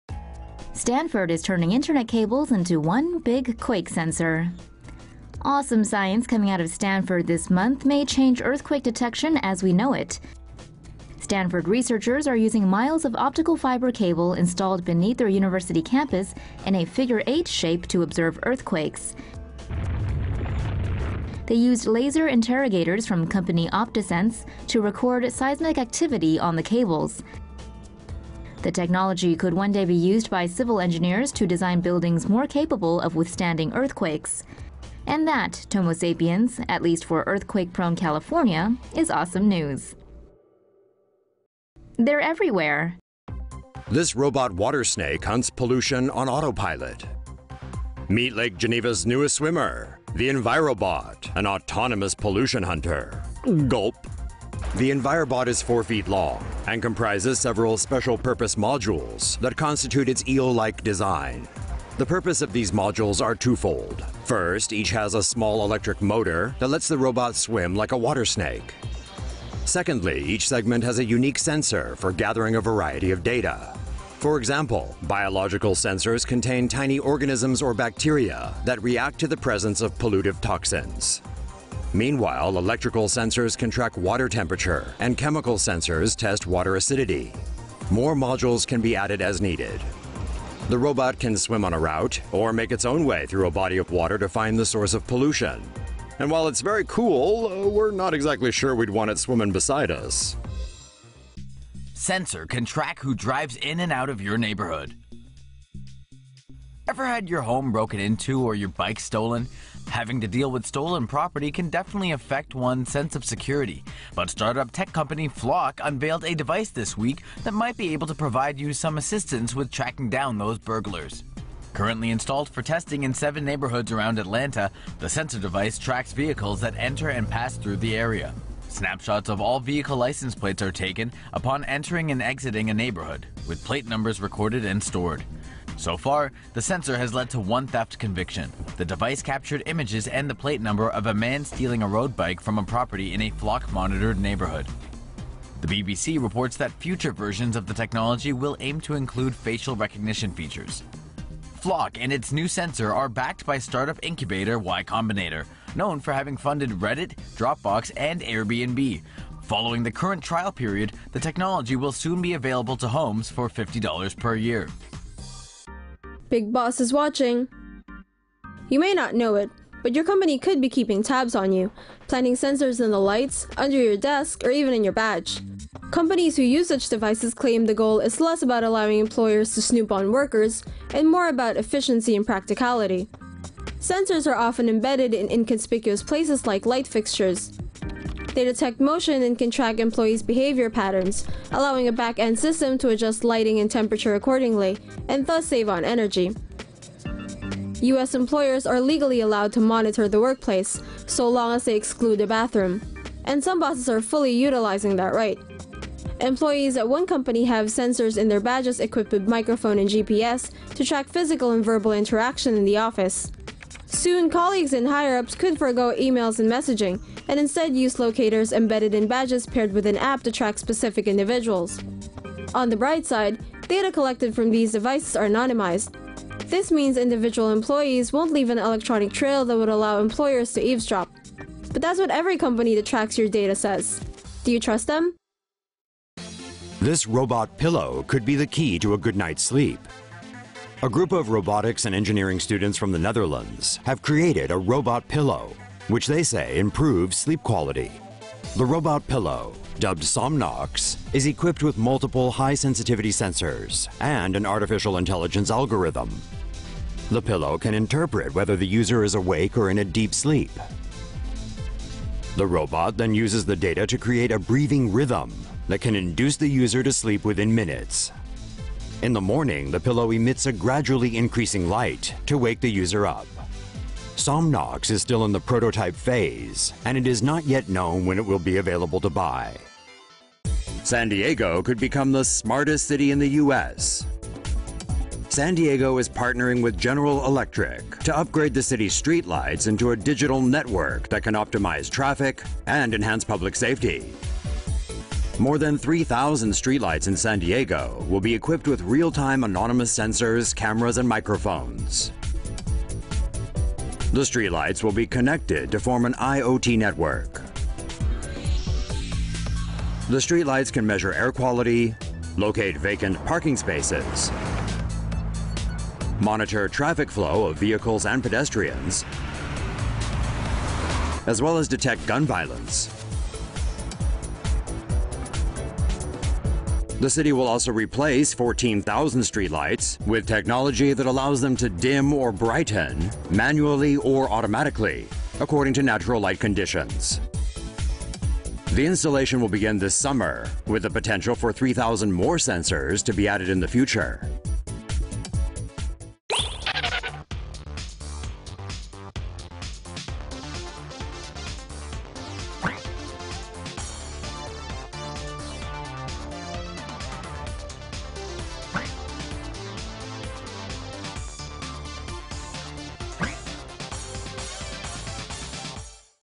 Earthquake sensors: Stanford's internet cables double as an earthquake sensors - TomoNews
SAN FRANCISCO — Researchers are using miles of optical cable beneath the Stanford university campus to observe earthquakes.

According to a news release from the university, scientists used laser interrogators from company Optasense to record seismic activity on the cables.

Stanford Geophysics professor Biondo Biondi says every meter of cable in the 3-mile long network acts as a sensor.

"We can continuously listen to — and hear well — the Earth using pre-existing optical fibers that have been deployed for telecom purposes," Biondi said.

The technology could one day be used by civil engineers to design buildings more capable of withstanding earthquakes.